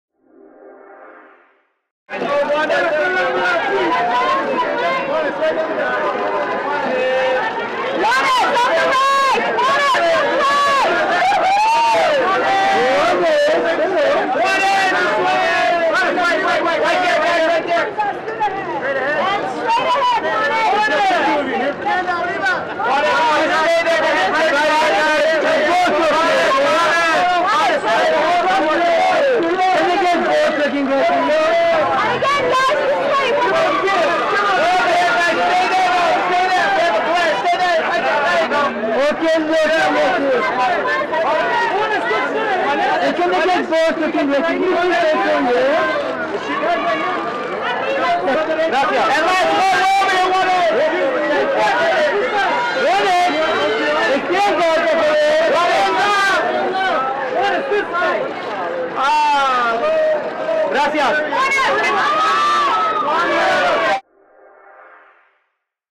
Juanes & Karen Martinez | McFarland, USA World Premiere | Red Carpet
Juanes and Karen Martinez arrive on the red carpet for “McFarland, USA” world premiere at El Capitan Theater in Los Angeles, Ca February 9th, 2015